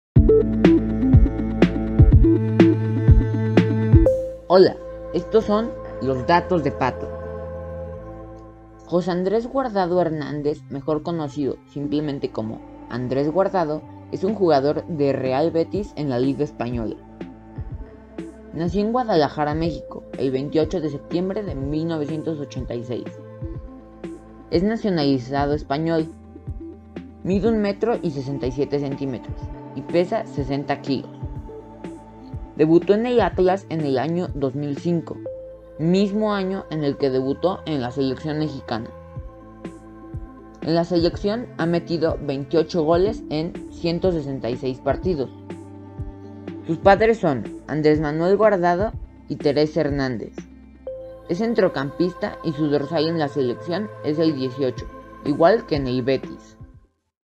Los datos de Pato|| Andrés Guardado
¿Eres seguidor de Andrés Guardado?
Te dejo unos datos para que lo conozcas un poco más ¿Sabes algun otro dato de él?